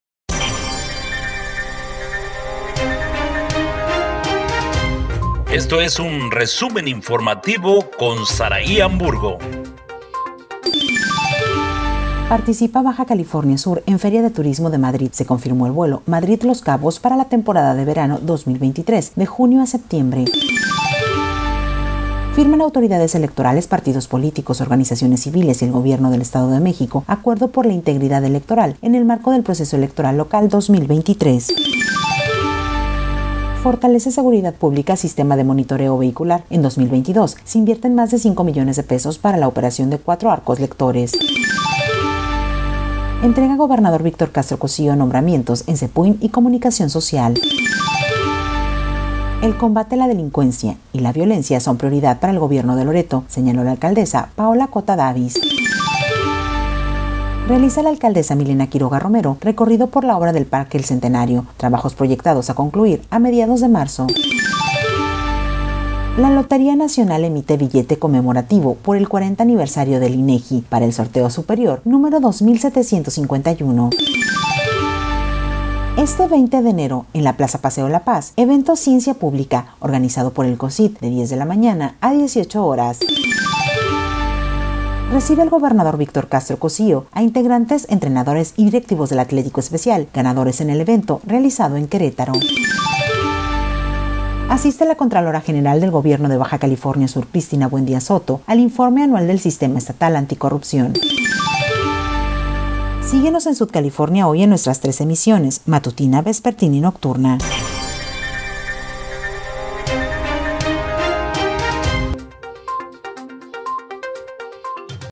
Resumen informativo - 20 de abril
Resumen informativo - 20 de abril en Baja California Sur con Zarahí Hamburgo.